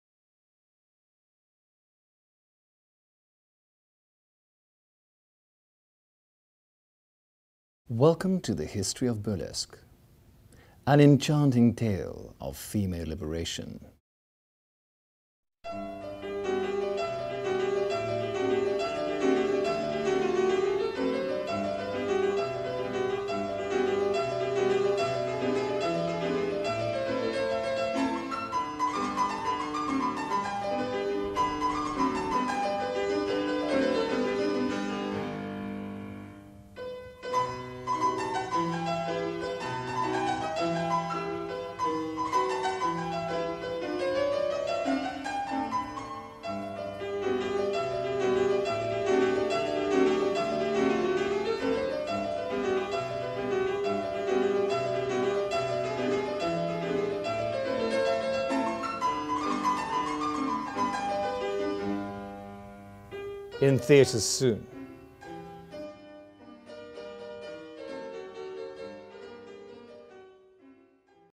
The History of Burlesque Film 2016
The History of Burlesque - An Enchanting tale of female Liberation.

An exciting 'history Lesson' by Collective Exceptionelle.

Collective Exceptionelle takes the visitor into the history of Burlesque.
In 4 scenes that are supported by authentic vintage style Burlesque performances and costumes we take the visitor on a trip through erotic history.
Where striptease is an easy way to turn on men….
Burlesque dance and theater is an art to liberate women, celebrate female power and practice the art of teasing!
The undressing is a metaphor for liberation….
Burlesque embraces women of all sizes, ages and cultures and therefor is a beautiful experience for both sexes….
Burlesque is also a way to make fun of yourself, relationships, environmental tendencies & politics and uses funny gimmicks to underline that.
With dazzling stories, music and images people will get the most fun, exciting and liberating, live art history lesson they could ever imagen and are invited to bring more adventure in their lifes.